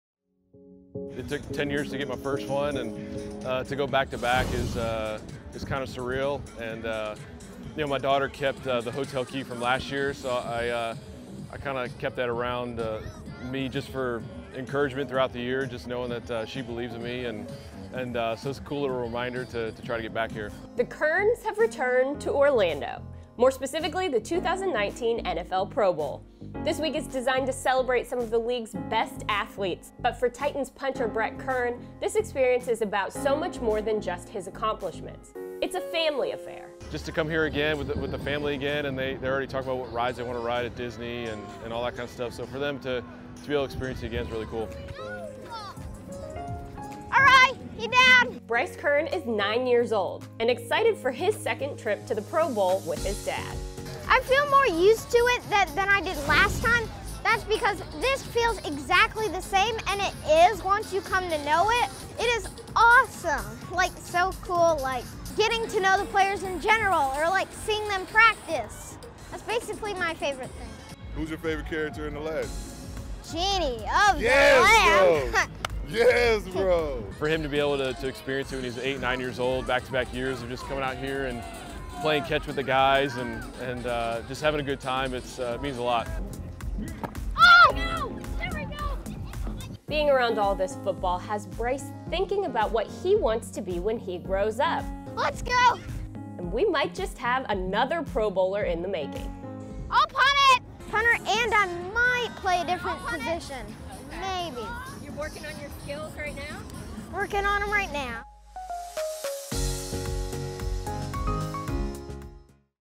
Like Father, Like Son: Bryce Kern's Pro Bowl Experience
The Pro Bowl is more than just a game for Titans punter Brett Kern, it's a family affair.